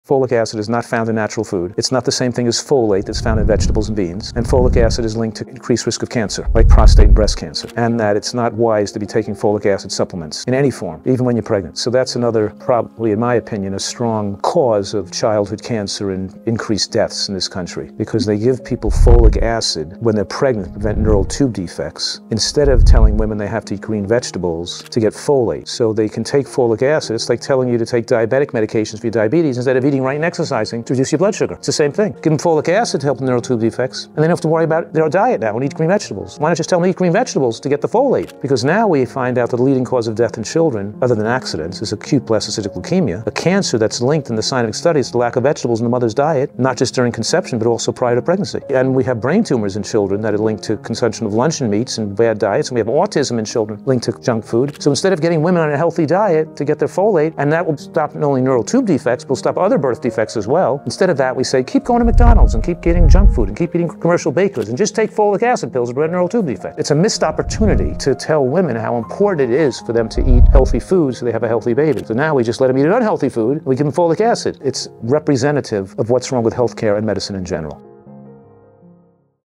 Folic Acid & Pregnancy: Could It Harm Your Baby?
Folic acid isn’t the same as folate, but most people don’t know the difference. It’s added to prenatal supplements, but could it be doing more harm than good? Before you take another pill, here’s what you need to know about the risks no one is talking about.

🥬🚀 Join my free, 5-day Nutritarian challenge, and I’ll change the way you think about food – forever.